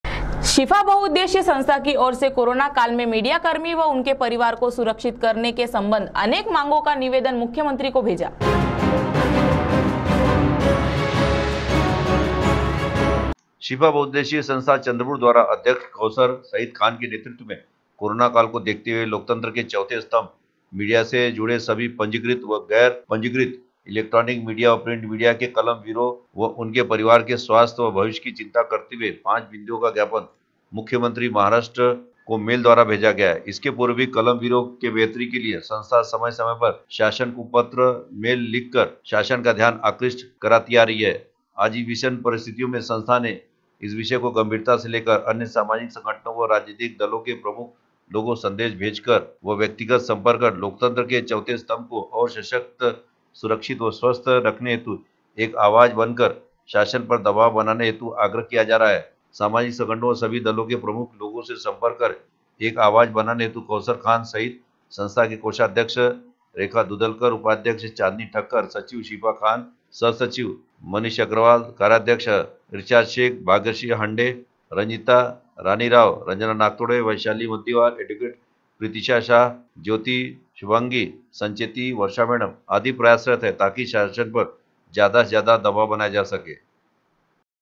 CTV News 21 April 2021 शिफा बहुउद्देशिए संस्था की ओर से कोरोना काल मे मिडियाकर्मी के परिवार के संब
शिफा बहुउद्देशिए संस्था की ओर से कोरोना काल मे मिडियाकर्मी के परिवार के संबंध मे मुख्यमंत्री को पत्र भेजा गया